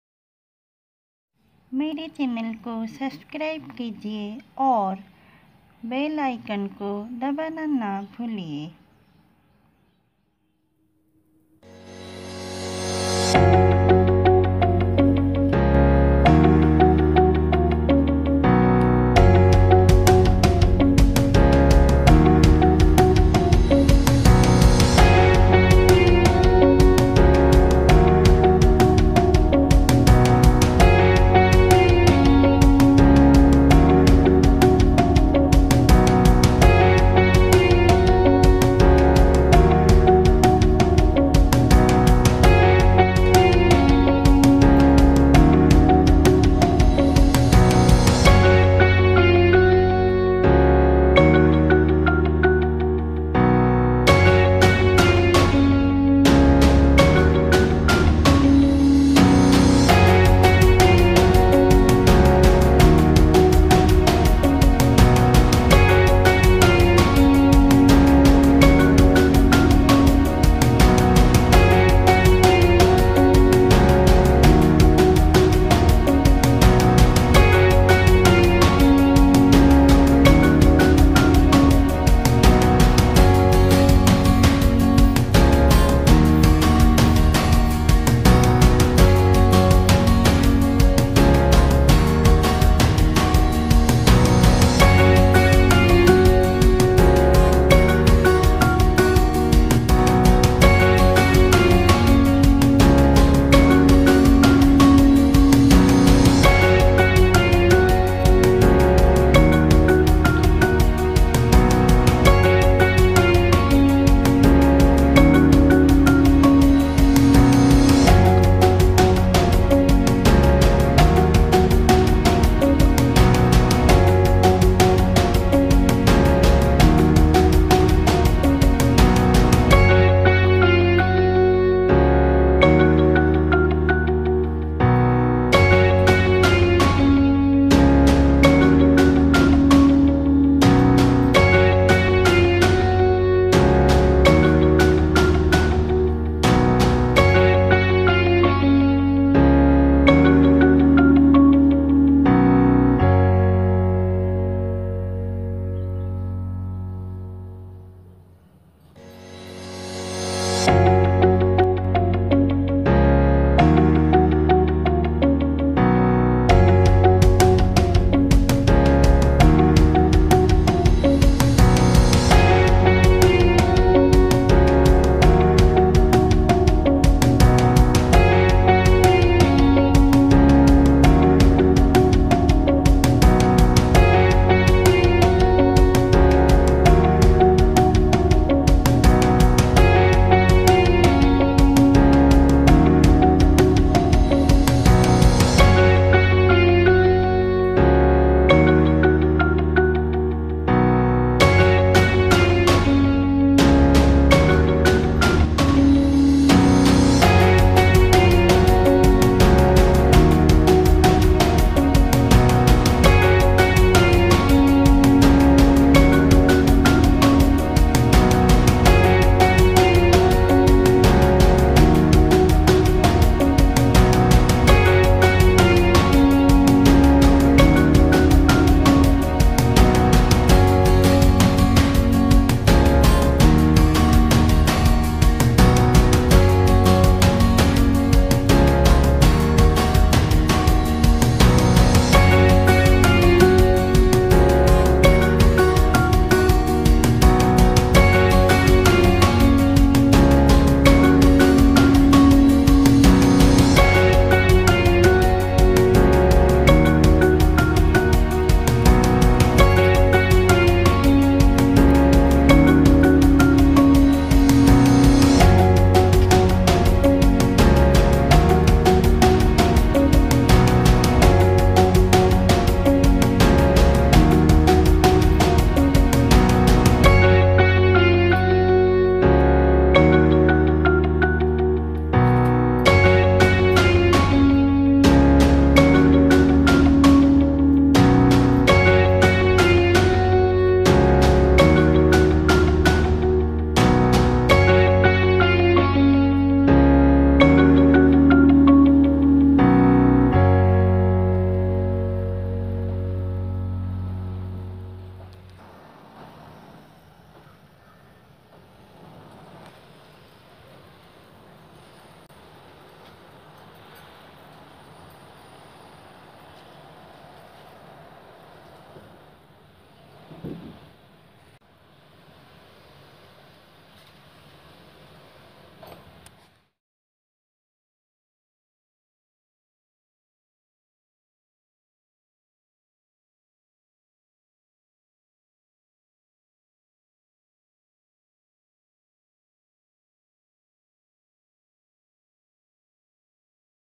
how to make lilly paper flower
Decorate your home with artificial Lili flowers made over paper more fresh and beautiful than original.